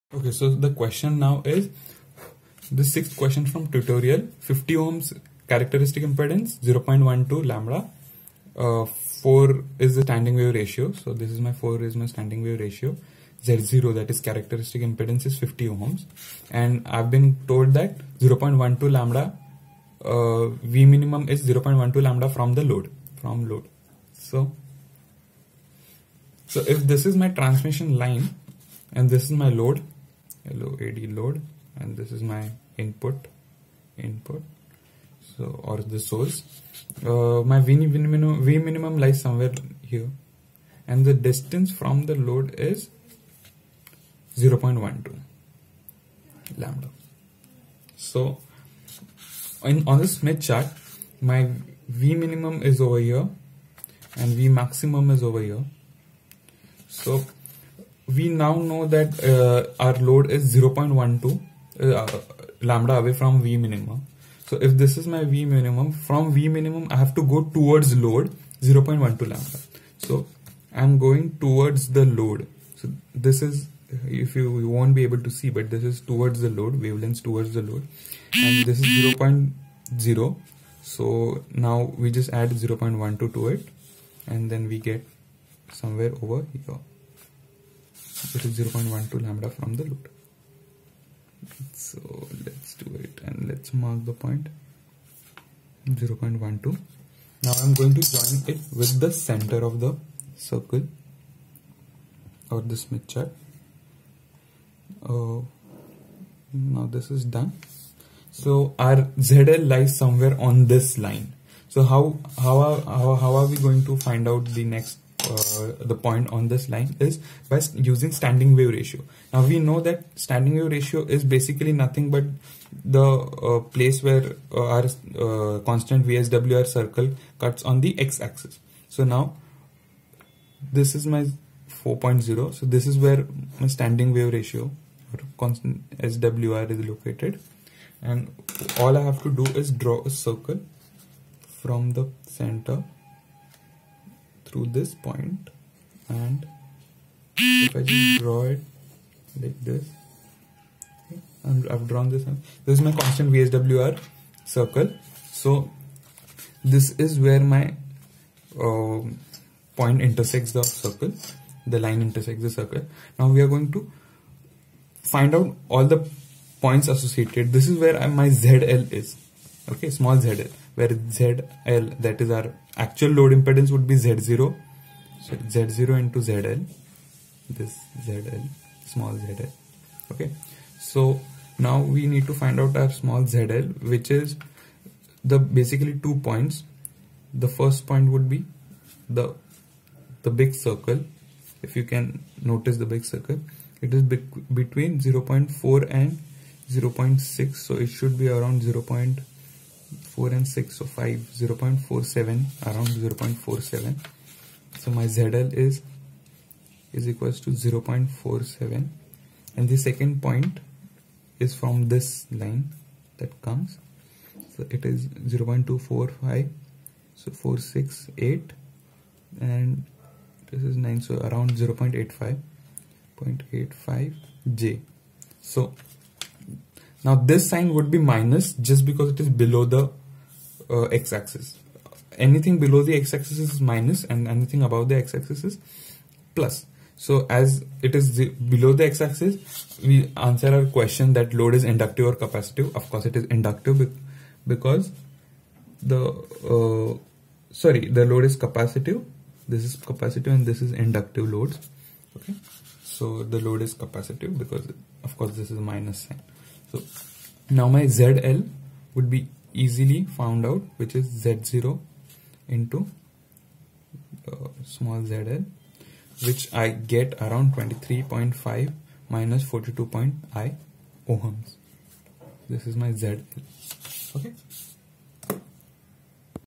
Smith Chart example #1
The question is taken from EME tutorial 4 question 6 .

6. The distance of the load from the first minimum voltage in a 50Ω line is 0.12λ and the
standing wave ratio is 4. Find the load impedance. Is the load inductive or capacitive?